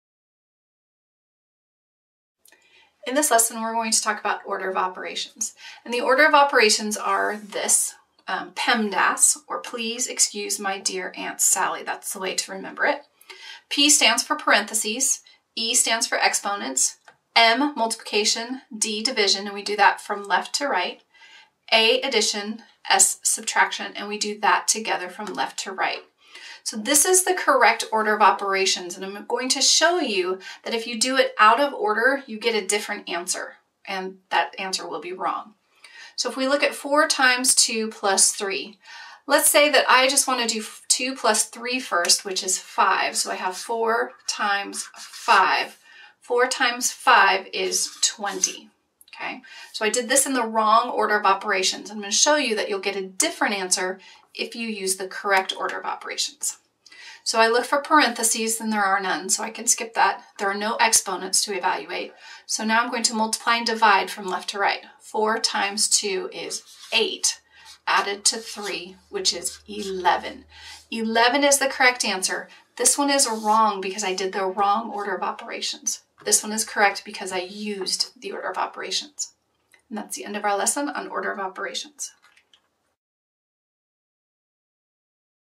Order of Operations in pre - algebra ( PEMDAS ) Video Tutorial For Kids
- Teach children pre-algebra with order of operations in equations. Children will learn which operations to first solve in algebra equations. Math video lesson for kids at home and in school. Prepare kids for Algebra I and Algebra ii by watching this video. See how the wrong order of operations can lead to the wrong results in a math problem.